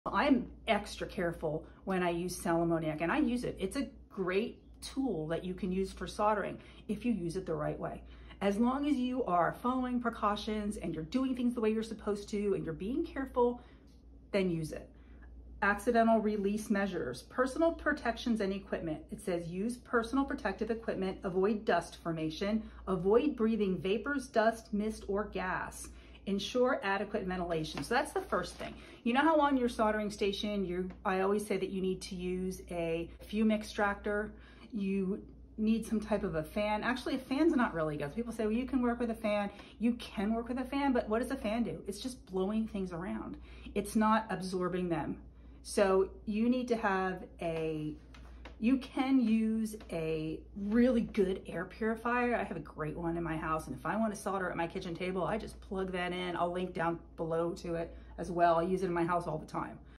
Sal ammoniac for soldering Safety part one ￼soldering
Using sal ammoniac safely - adequate ventilation is essential.